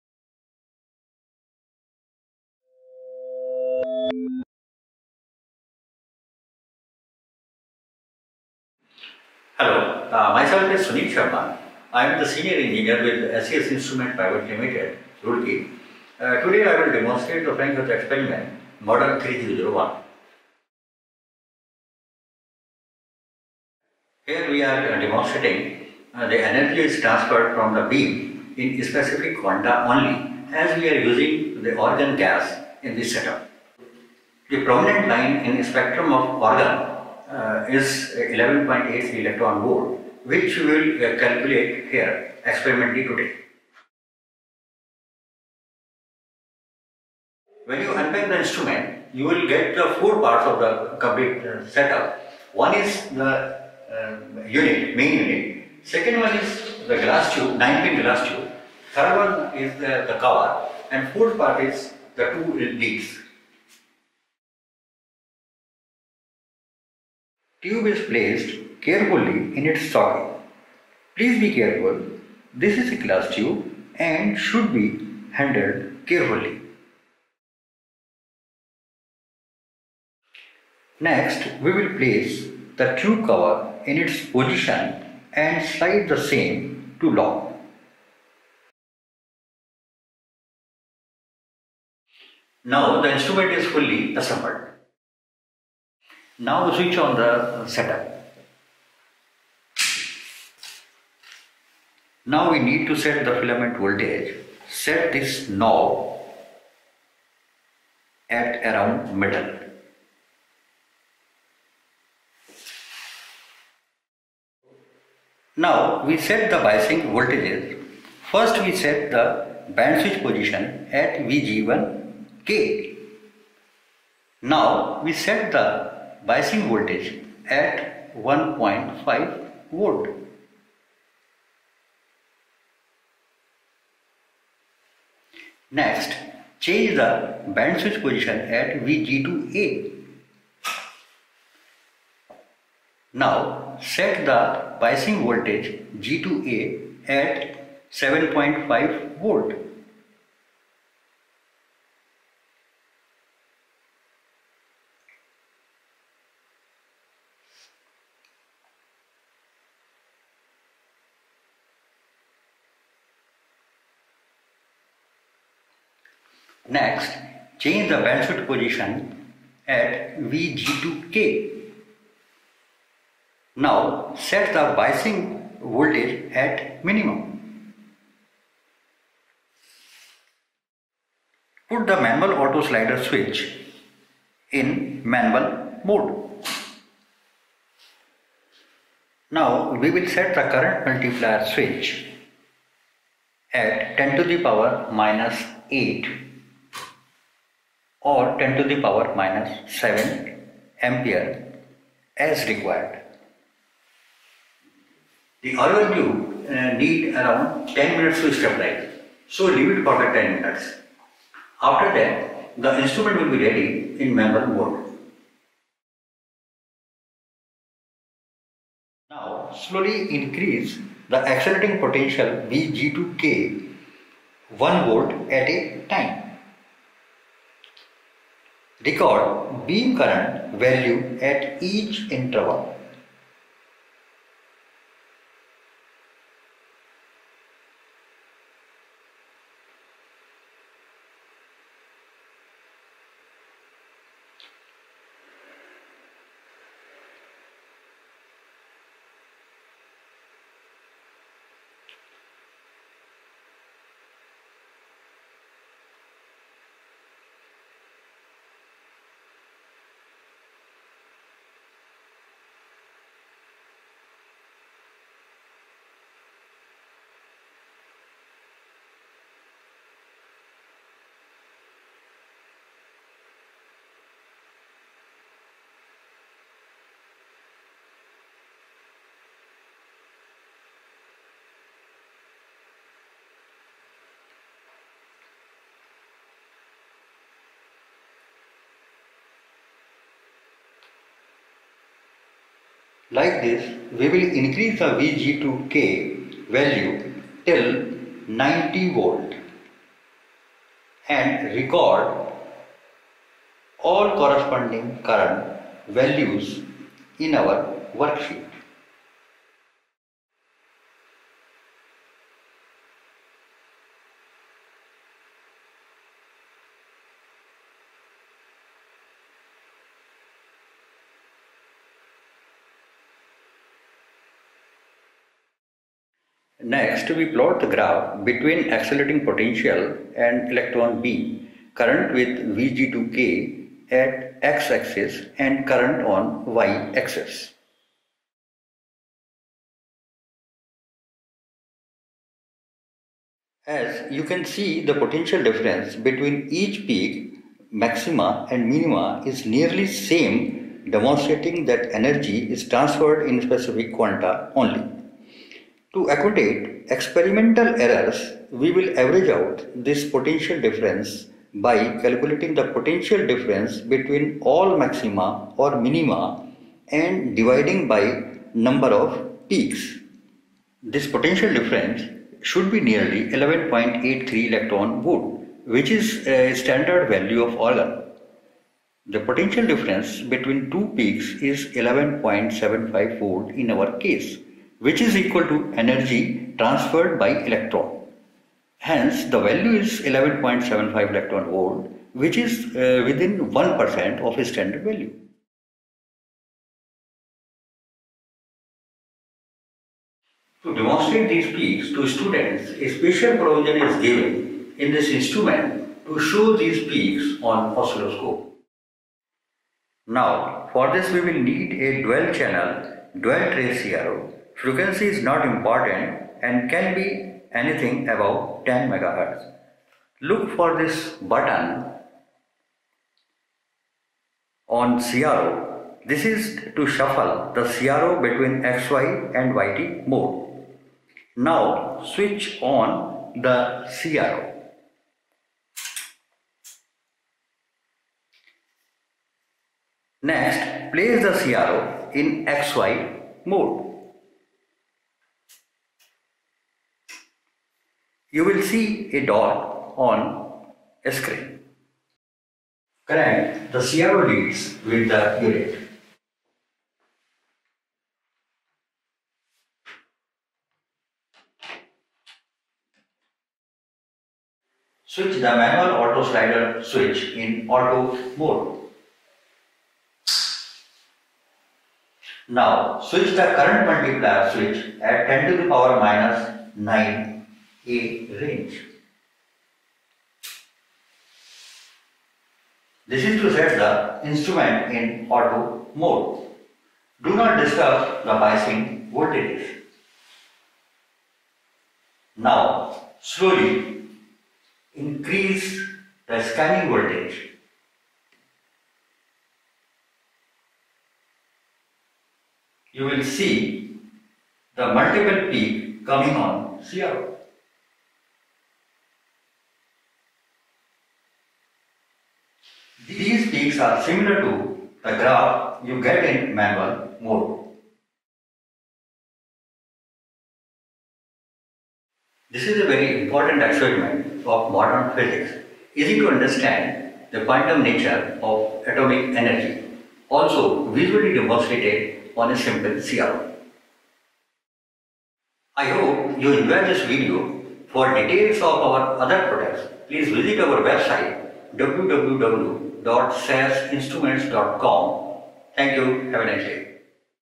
Frank Hertz Experiment, FH-3001
Demonstration video of Frank Hertz Experiment, Model FH-3001, a product of SES Instruments Pvt Ltd. Roorkee, India